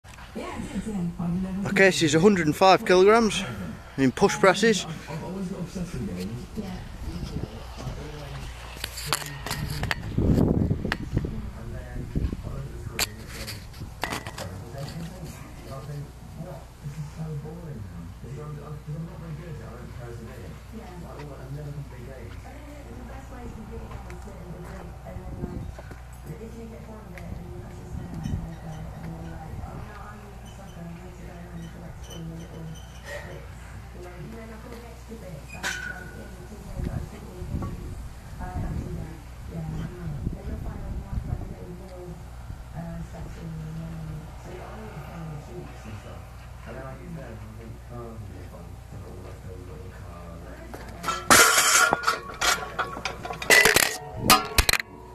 push press (230 x 5) 105 kg x 5
push press 105 kg x 5 nearly snapped my back on the last rep!!